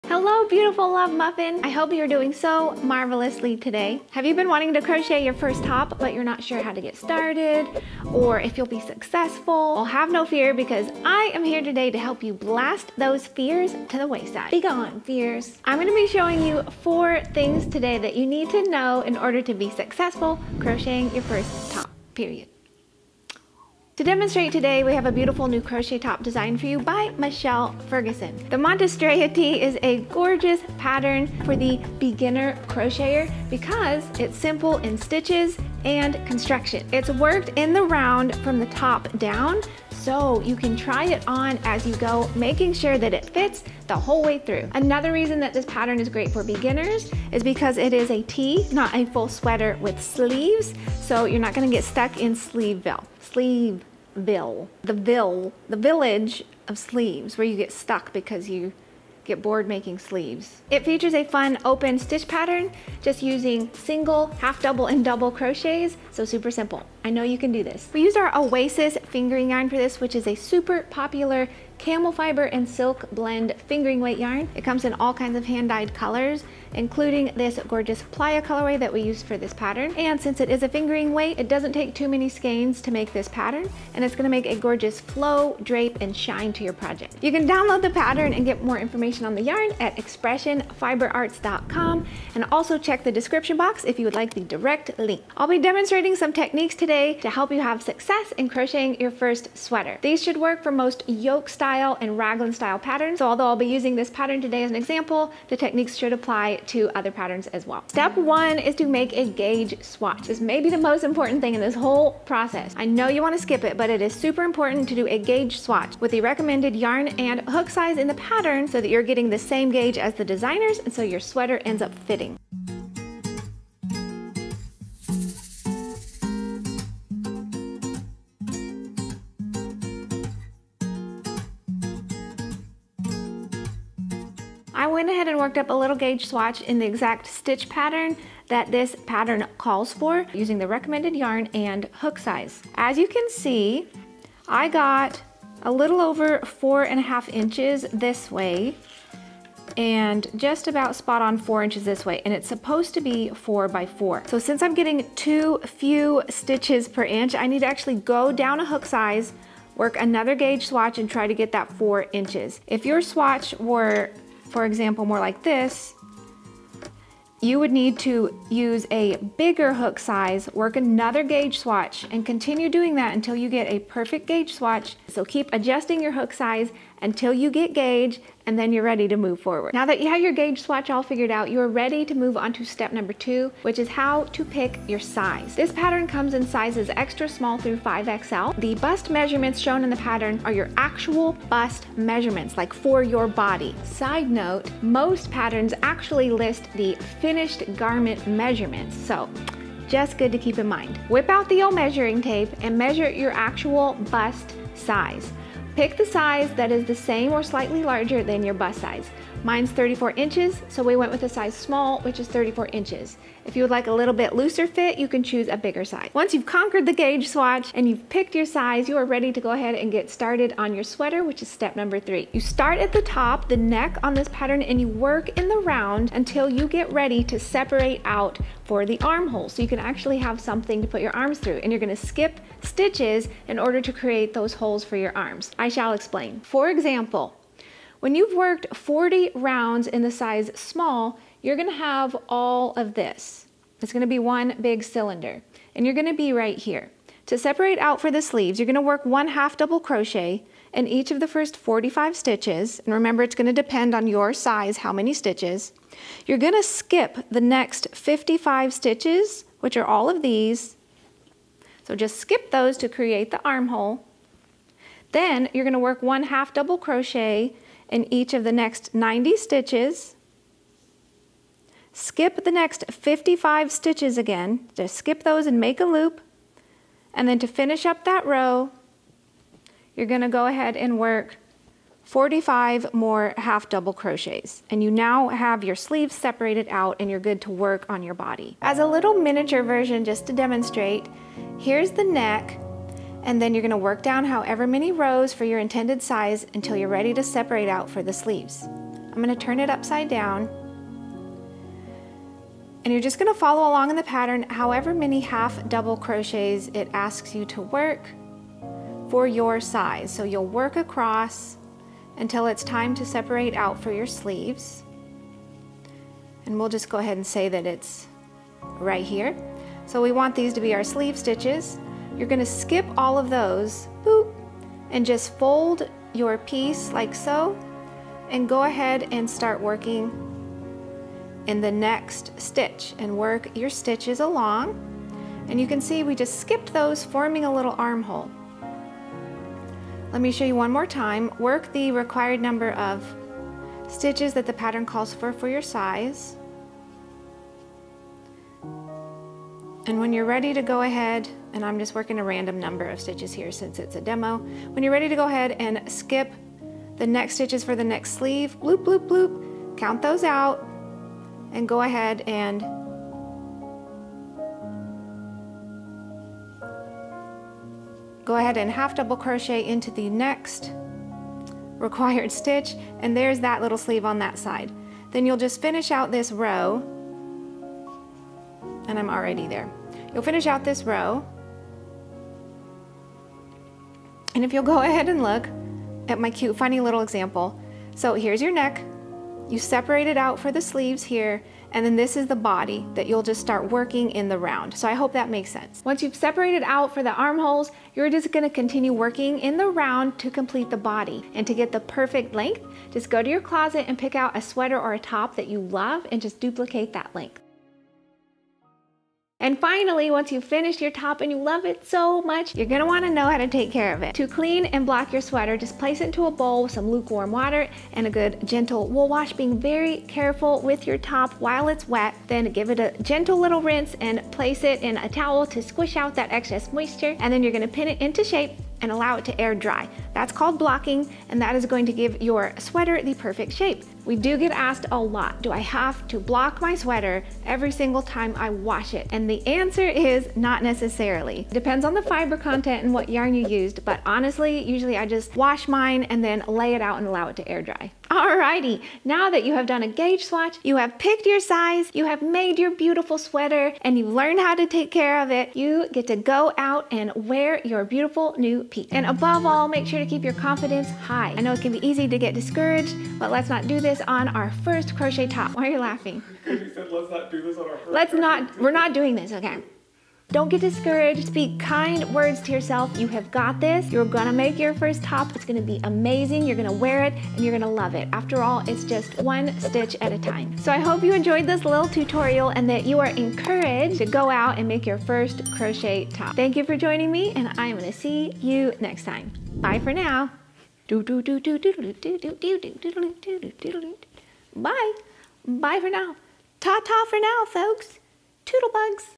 Afraid to Crochet Your First Sweater? Lose those fears and let’s crochet a top!!
Hey, sweet soul. Crocheting a sweater or tee or tank can be soooo exciting! You actually MADE a garment. It's incredible.
But sometimes bigger projects like this can feel overwhelming or intimidating. Let's break it down in today's video and learn some tips on how to crochet your first sweater or top. Hurray!

Find us here:
Also on Ravelry, Pinterest and Twitter, we're ExpressionFiber

“You can’t cross the ocean of life just by dreaming about it, you have to jump in and swim.” – Debasish Mridha